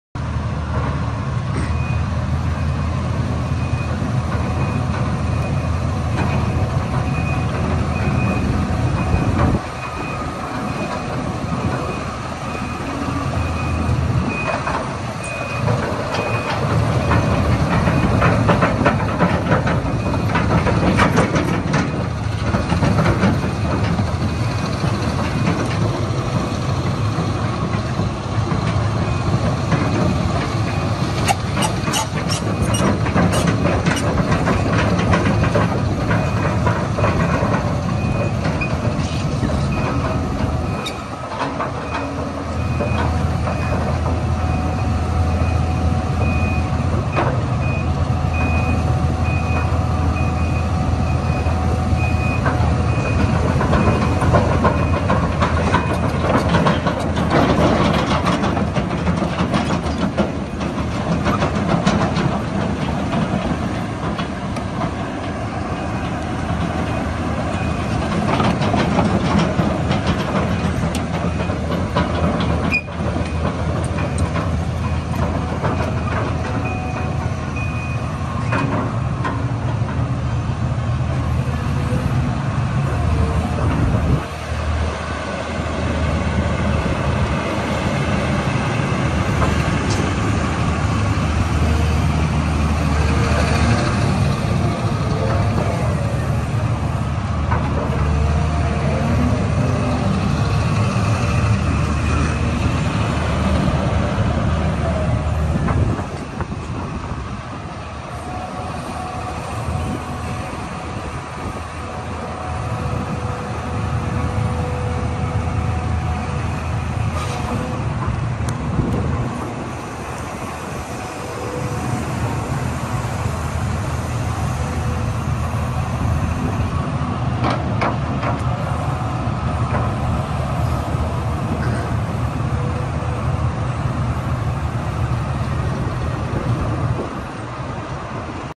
2012 CASE CX350C For Sale
60 in bucket, mechanical thumb, 13 ft stick, A/C cab, rear view camera